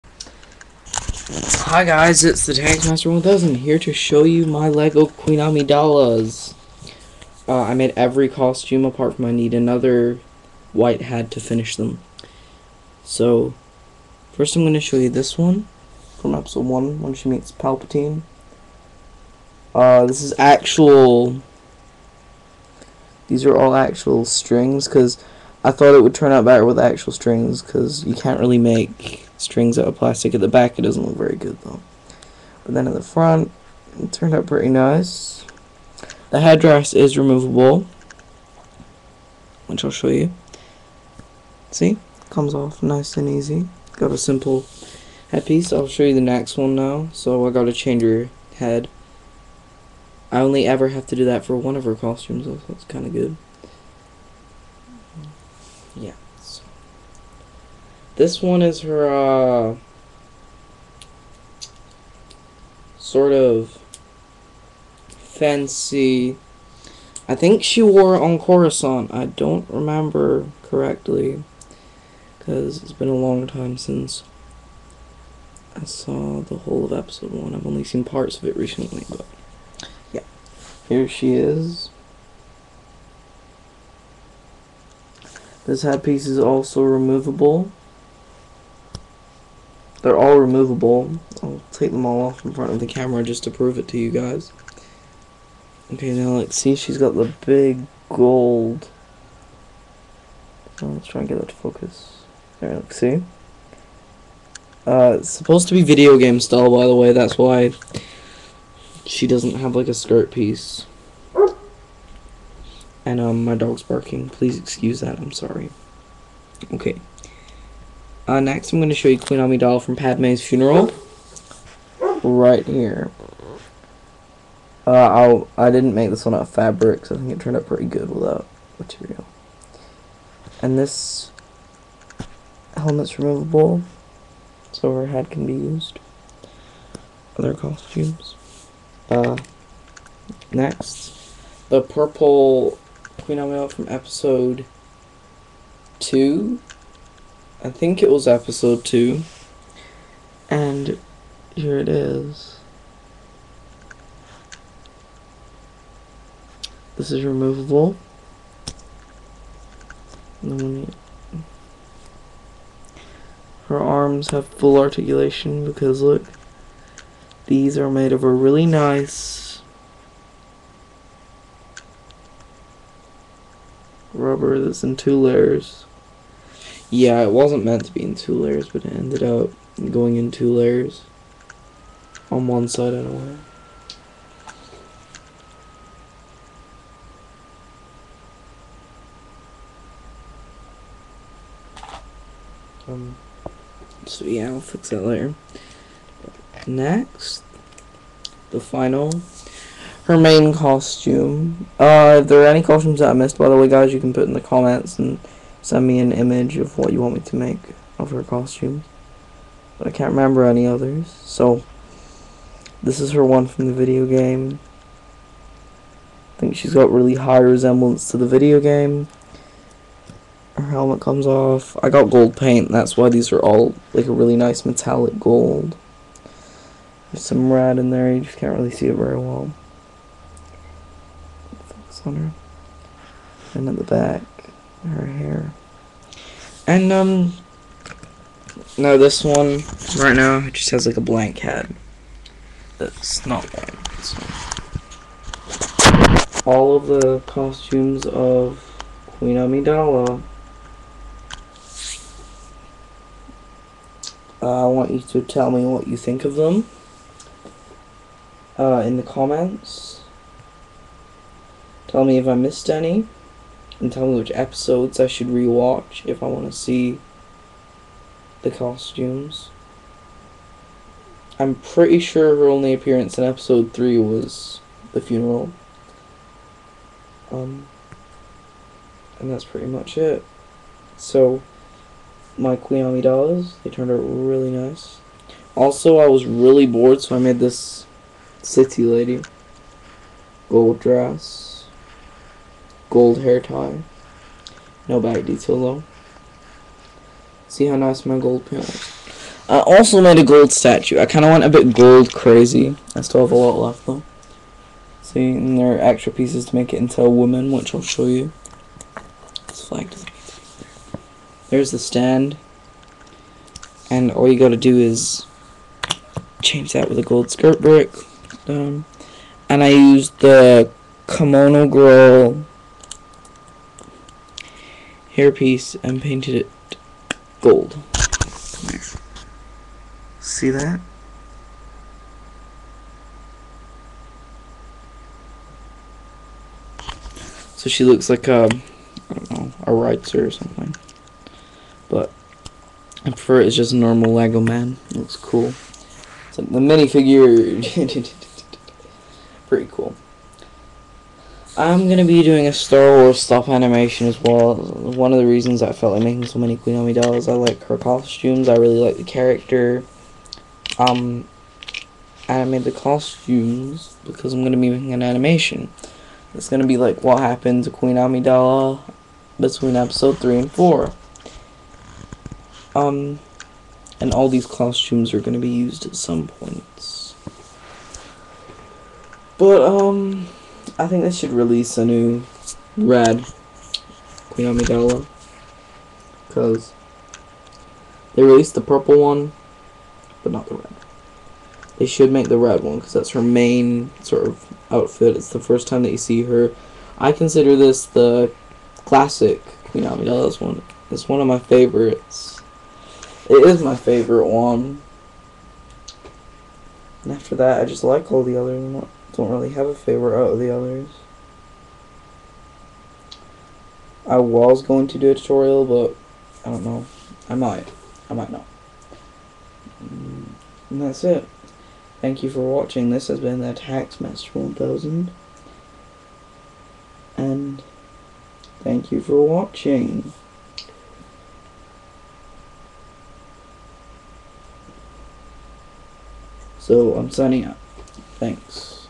TheTaxmaster10o0's queen amidala costumes
lego queen amidala video game style but with all costumes
all head dresses are removable if u want to buy one send me a lego kimono girl and i can make any one of them